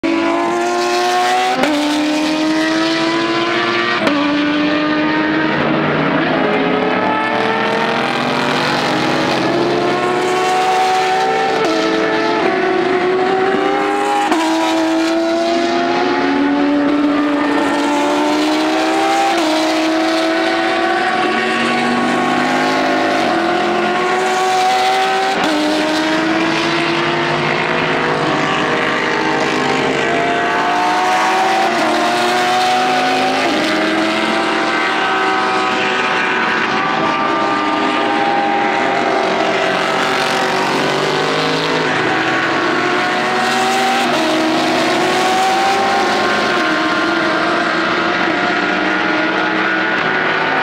2012 Roar Before the Rolex 24 Hours
A quick video of some racing action of the final practice before the 24 Hours of Daytona